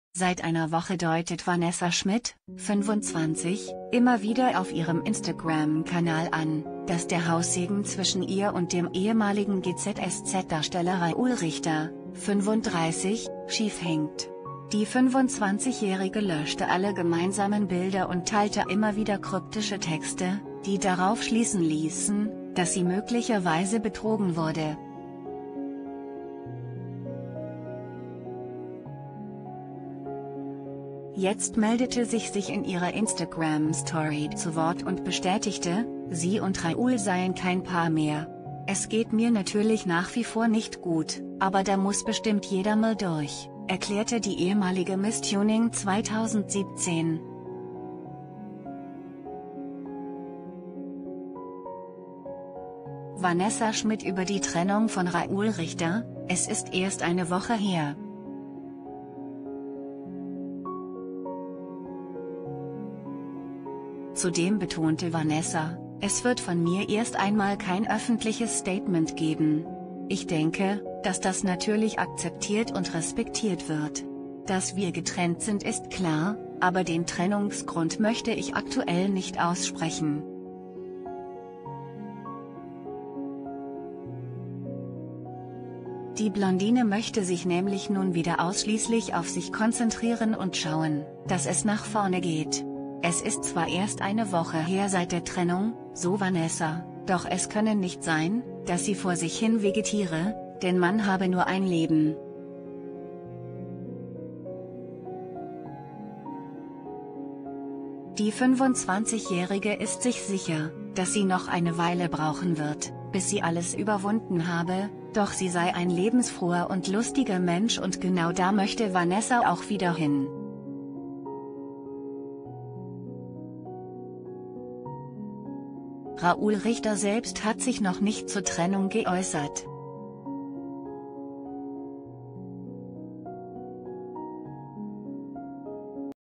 Raúl Richter & Vanessa Schmitt: Jetzt bestätigt sie die Trennung
Seit einer Woche deutet Vanessa Schmitt (25) immer wieder auf ihrem Instagram-Kanal an, dass der Haussegen zwischen ihr und dem ehemaligen GZSZ-Darsteller Raúl Richter (35) schief hängt. Die 25-Jährige löschte alle gemeinsamen Bilder und teilte immer wieder kryptische Texte, die darauf schließen ließen, dass sie möglicherweise betrogen wurde.
Jetzt meldete sich sich in ihrer Instagram-Story zu Wort und bestätigte: Sie und Raúl seien kein Paar mehr. "Es geht mir natürlich nach wie vor nicht gut